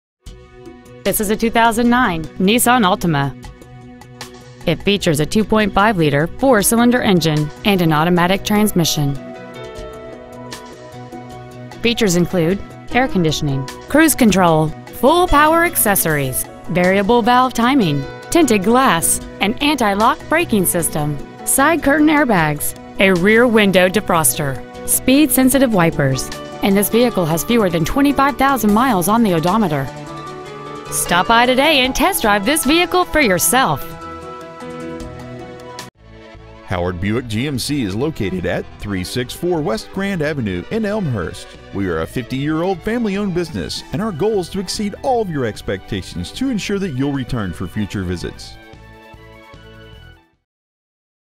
2009 Nissan Altima Elmhurst IL
Year : 2009
 Make : Nissan
 Model : Altima
 Engine : 2.5L 4 cyls
 Trans . : Automatic
 Exterior : Black
 Miles : 24,000
 Interior : Blonde w/Cloth Seat Trim
 Stock : 2540P

 364 W. Grand Avenue